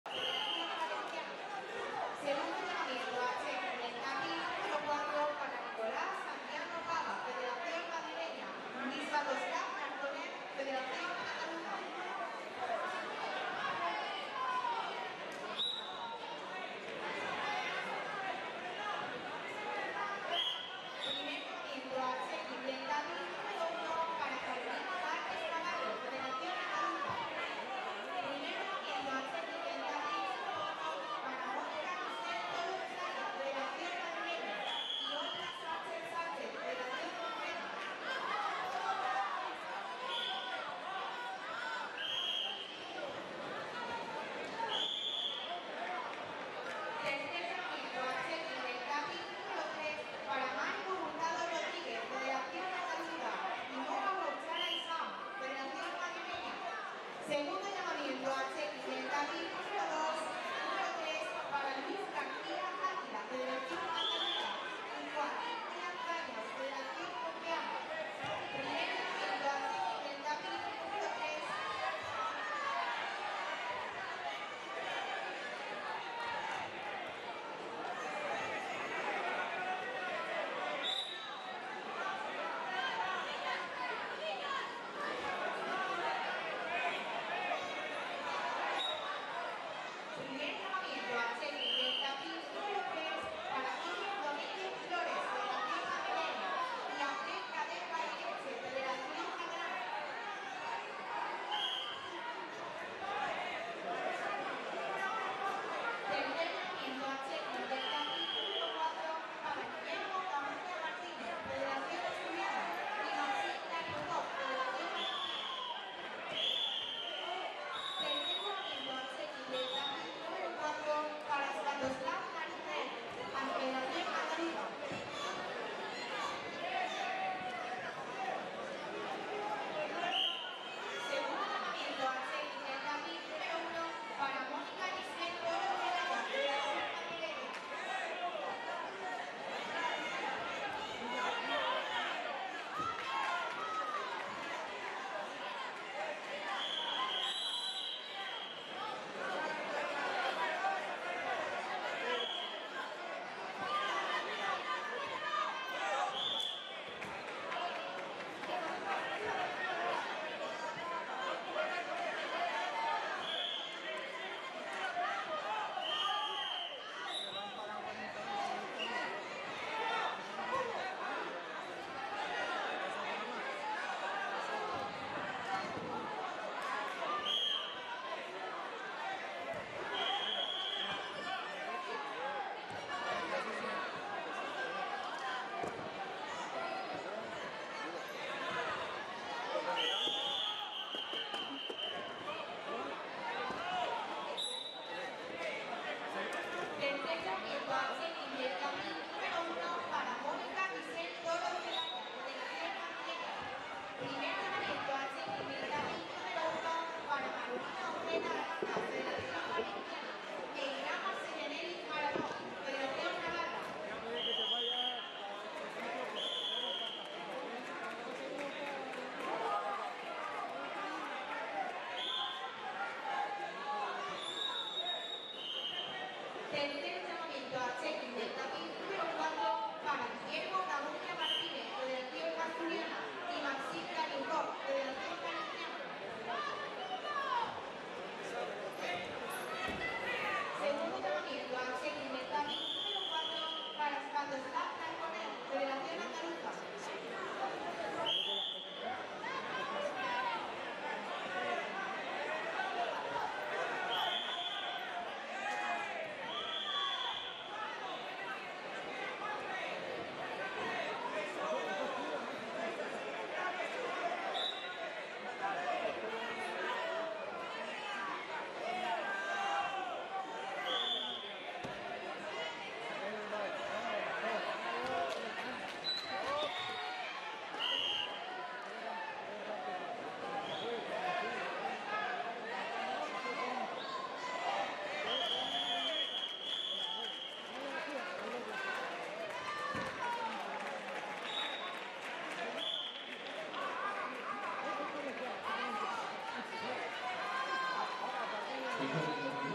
💥 LO - GIL FUEYO, HUGO (GAL) BUKOTKO, STEFAN (MUR) 💥
El campeonato de España U15 de Luchas Olímpicas (libre femenina, grecorromana y libre olímpica masculina) del 2023 fue un auténtico espectáculo deportivo con record de participación en las tres modalidades. Todas las autonomías estuvieron muy bien representadas por estos jóvenes deportistas que darán muchas alegrías a nuestra nación.

Celebrado en Carmona (Sevilla) en un fin de semana cargado de emociones y gran nivel de combates desde el primero hasta el último.